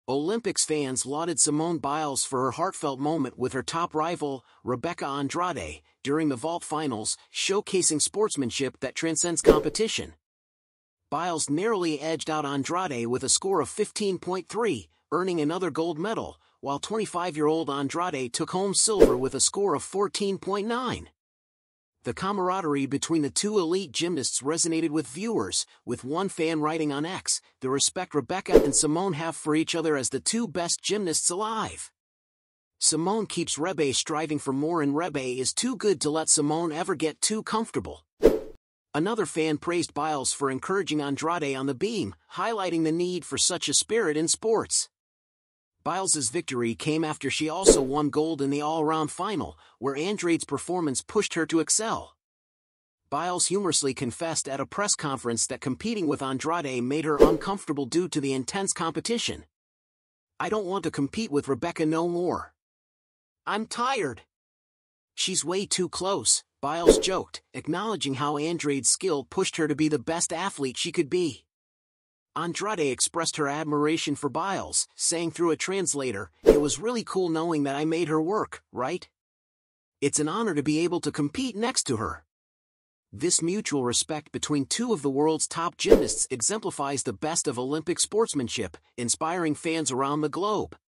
Fans Adore Simone Biles' Touching Post-Vault Moment with Rival
Olympics fans lauded Simone Biles for her heartfelt moment with her top rival, Rebeca Andrade, during the vault finals, showcasing sportsmanship that transcends competition. Biles narrowly edged out Andrade with a score of 15.3, earning another gold medal, while 25-year-old Andrade took home silver with a score of 14.9.